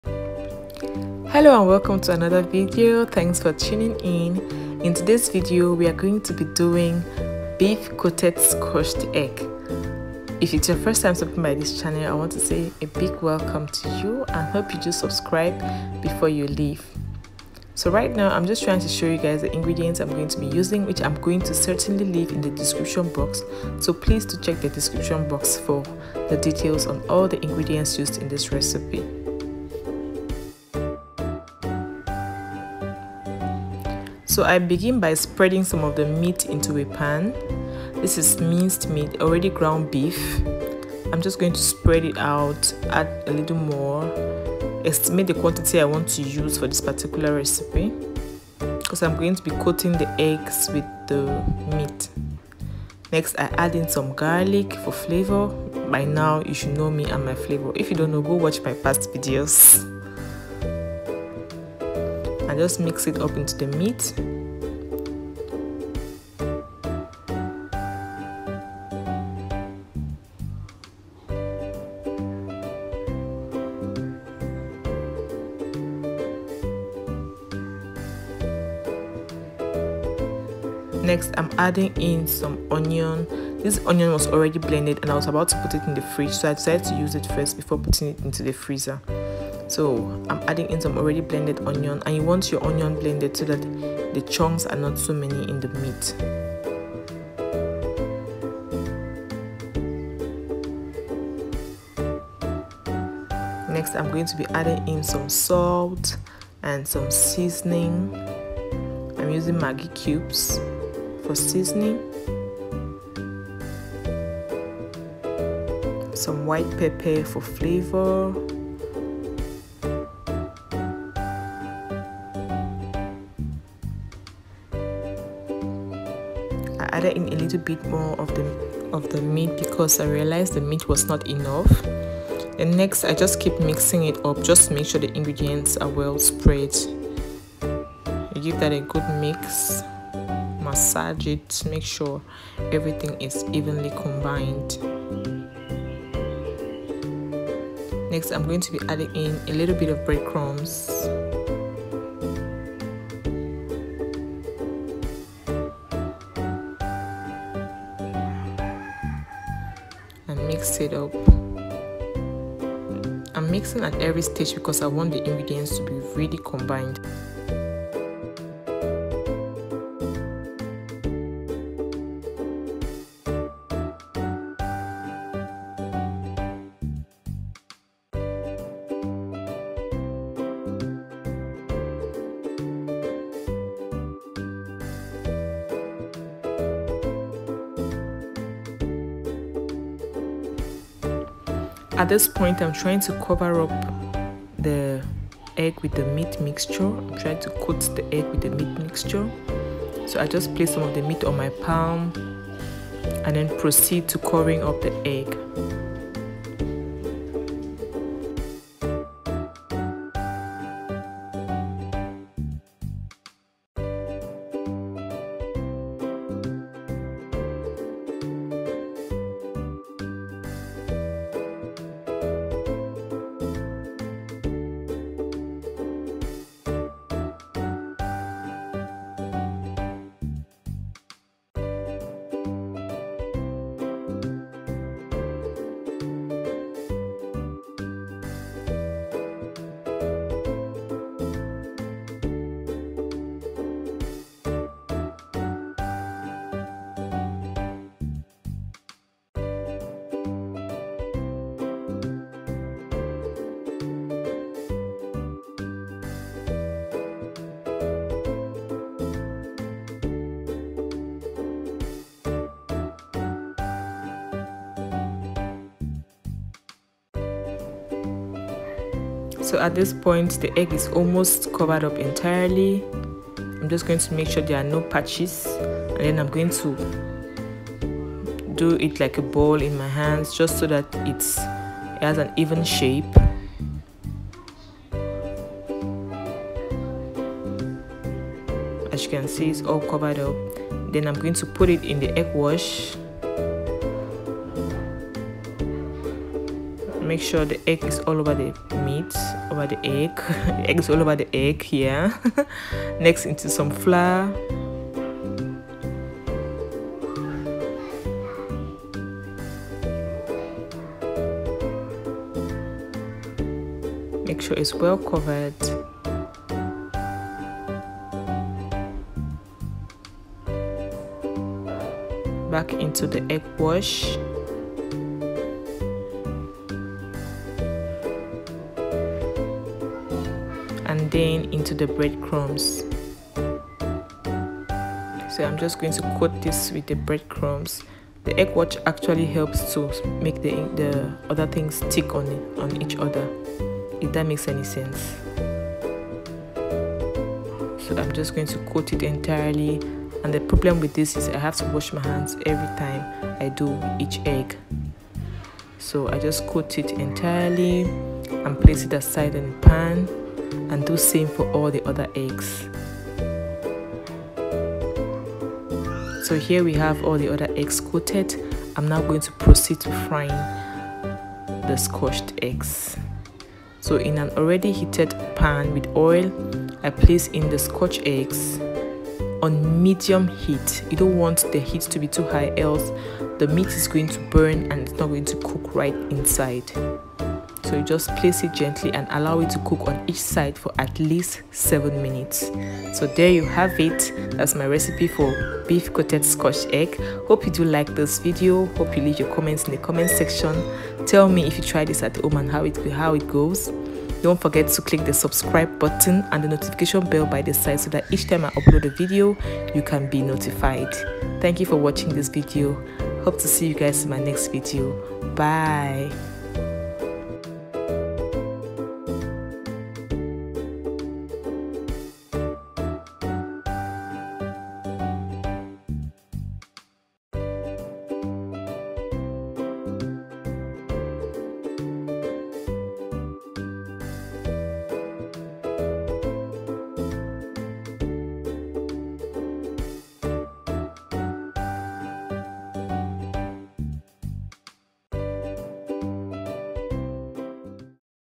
BEEF SCOTCHED EGGS RECIPE || CAMEROONIAN SNACK🇨🇲
Hello guys!!! This video features my recipe for beef coated scotched eggs! Hope you enjoy this video

Ingredients used

1/2 kg minced beef
4 boiled eggs
1 cup bread crumbs
1 cup of flour
2 whisked raw eggs
1 Tspn salt
1 seasoning cube
1 Tspn chopped garlic
1 Tspn white pepper
1 Tablespoon blended onion